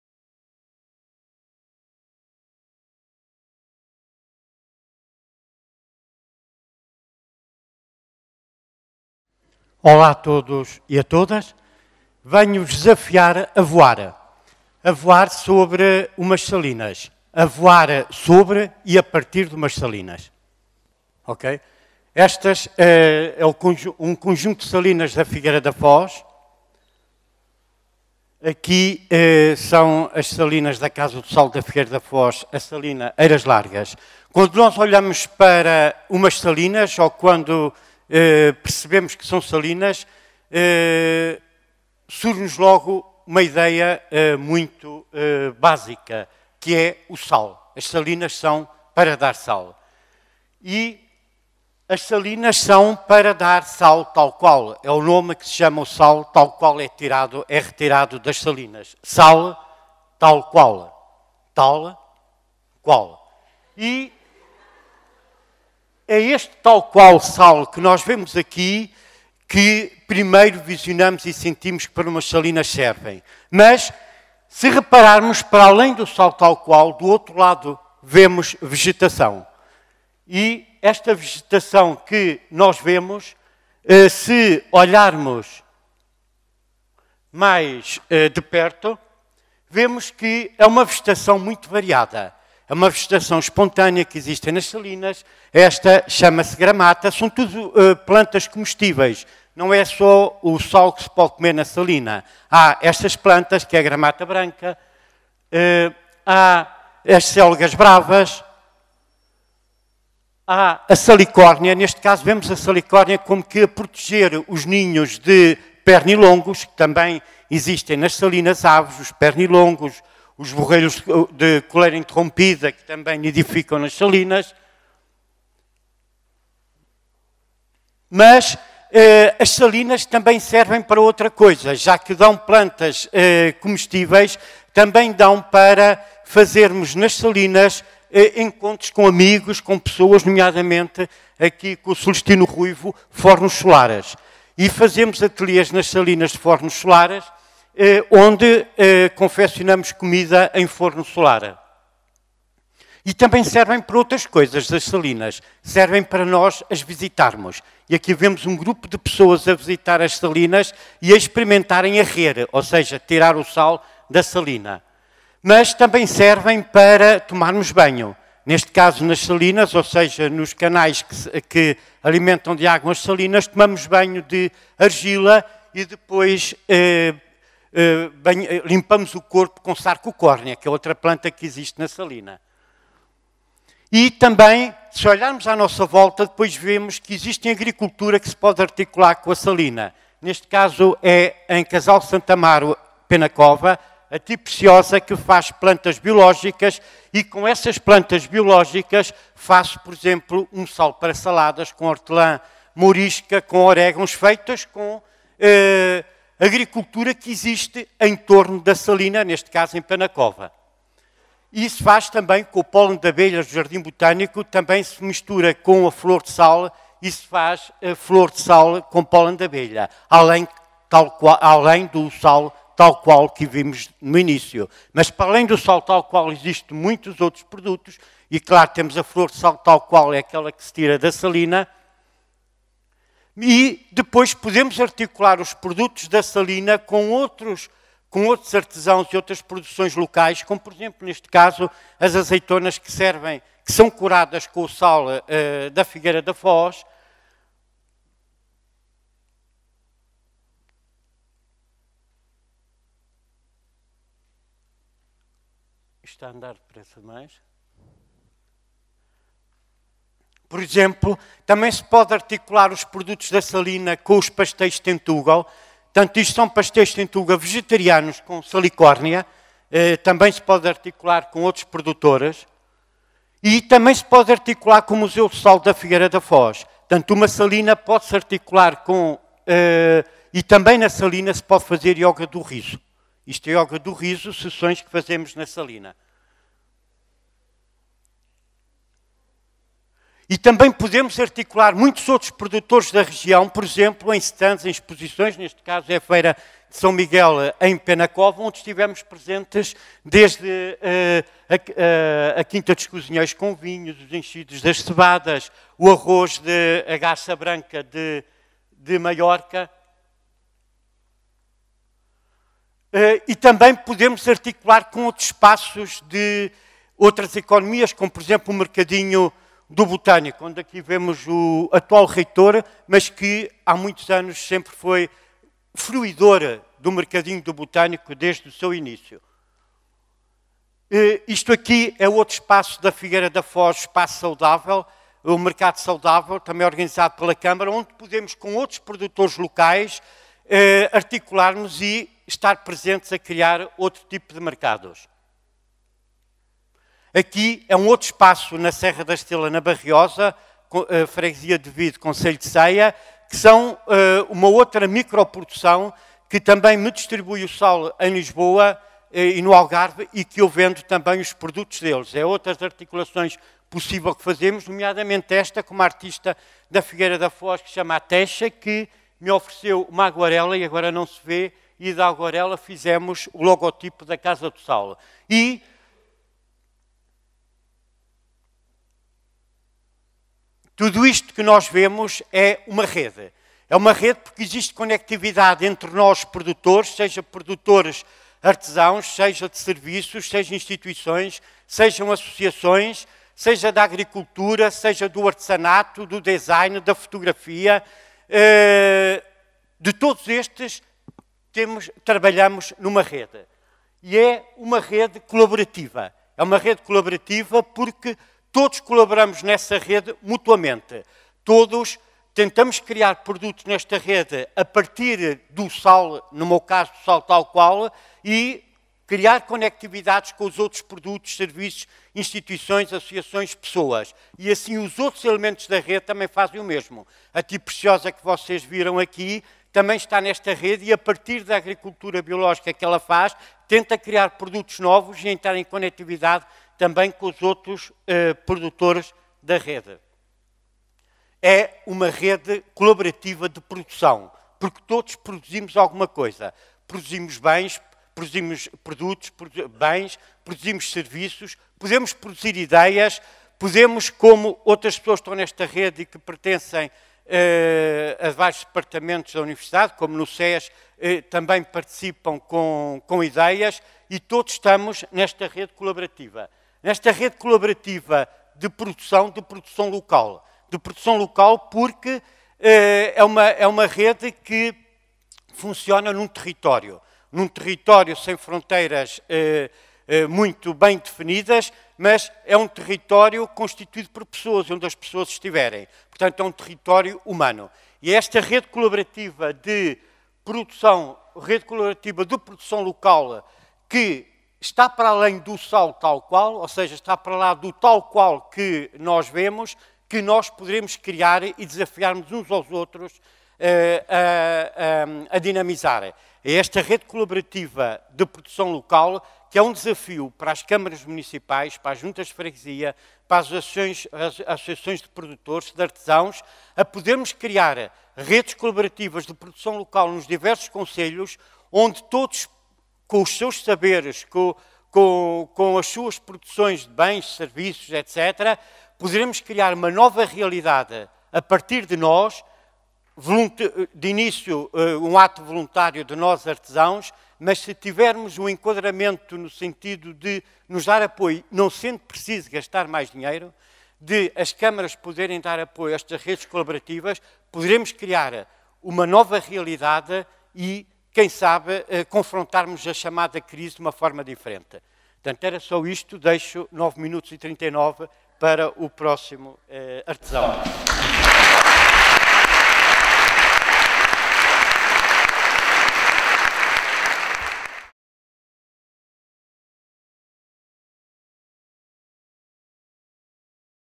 Sal Tal Qual: José João Rodrigues at TEDxCoimbra
José João Rodrigues é Agente de Desenvolvimento em projectos de empreendedorismo social e um grande promotor da salinicultura da Figueira da Foz em eventos por todo o país e no estrangeiro.
Das actividades profissionais que desenvolve destacam-se a dinamização do Projecto Casa do Sal da Figueira da Foz, projecto que cria e explora produtos e serviços ligados à salinicultura; o apoio ao desenvolvimento de actividades artesanais ao nível do marketing: criação, produção e comercialização; e a participação como orador em diversos seminários, congressos e colóquios.
É ainda Formador e Animador de Oficinas de Teatro do Oprimido dirigidas a técnicos, formadores e pessoas em situação de exclusão social.

Frequentou o Curso de Filosofia da Faculdade de Letras da Universidade de Coimbra e o Curso de Agentes de Desenvolvimento Local. Participa activamente em diversas associações, nomeadamente o GAF -- Grupo Aprender em Festa, a ANIMAR -- Associação para o Desenvolvimento Local e a ANDC -- Associação Nacional de Direito ao Crédito, da qual foi Cofundador.